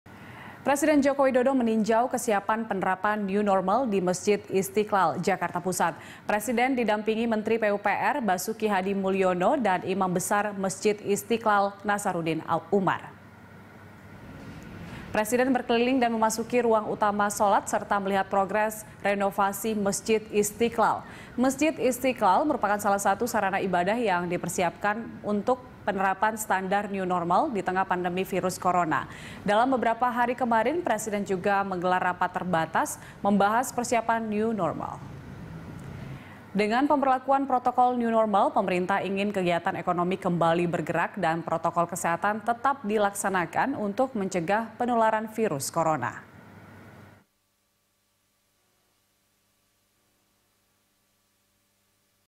Jokowi Tinjau Kesiapan Penerapan New Normal di Masjid Istiqlal
Presiden Joko Widodo meninjau kesiapan penerapan new normal di Masjid Istiqlal, Jakarta Pusat.